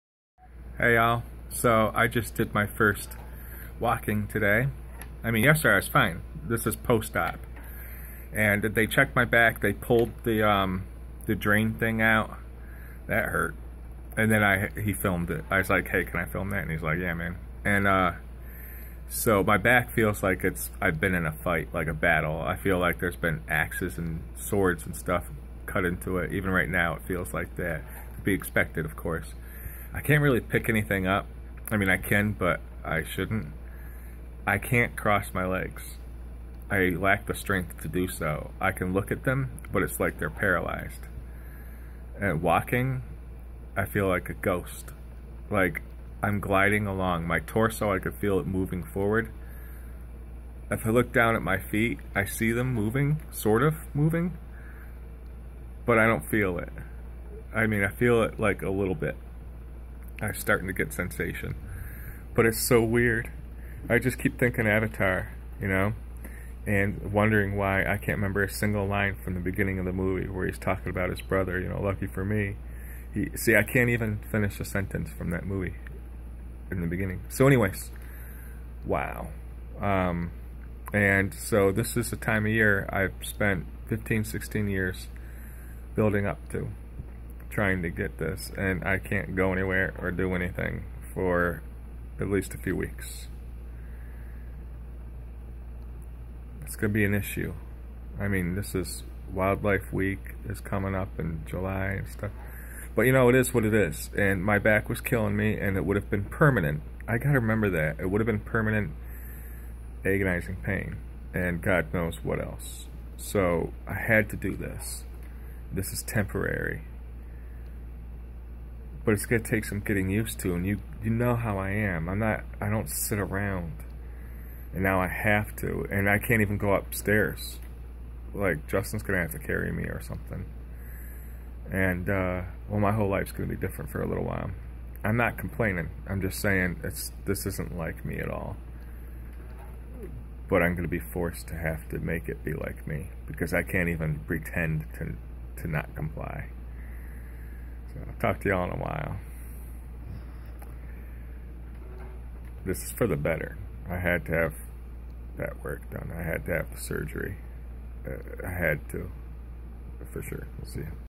June 18, 2020 Learning to walk? Didn't expect that.
So yeah, I need to take it slowly. like real slow. I may have just lost my summer. I want to cry if that's the case. I have lost every season and desire I have reached for for years and years. finally, I have a set of wheels coming my way and I am about to come to life. Covid killed money, camping and I don't want to list things but I said do or die, I am taking freedom to do things. no matter what, I cannot die anymore. But this surgery has hit at a troubling time. I cannot drive now, or buy a car. My dog is a hundred miles away, I cannot lay upon the grass, under the stars, and I simply cannot say more without trouble. I know I could have been permanently in 100 % pain and worse, but I cannot help but to fear so much. I have felt my time is short and the time is now but now, just up and walked out. you know what? screw it, I have always been defiant. I am walking after it, even if it kills me. I cannot live in limbo without being a ghost.